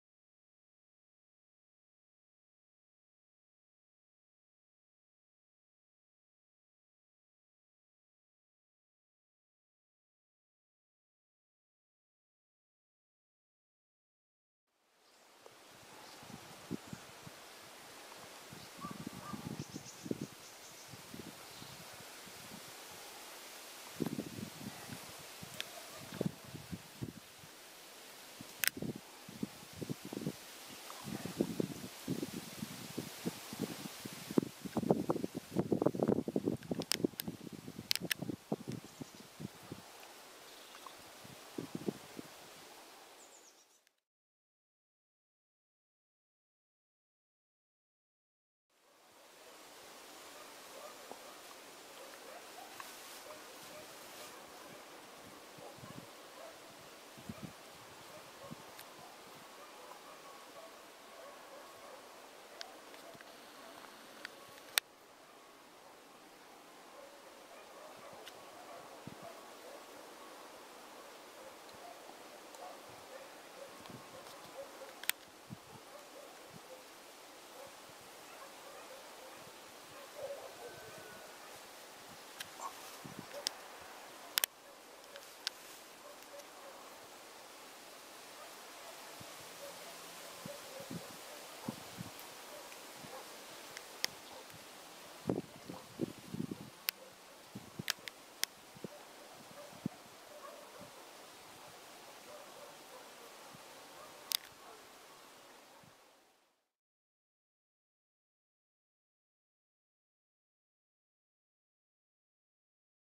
Горный Крым, посёлок Соколиное, встреча с белкой.
Горный Крым, прогулка по лесу,  встреча и знакомство с белкой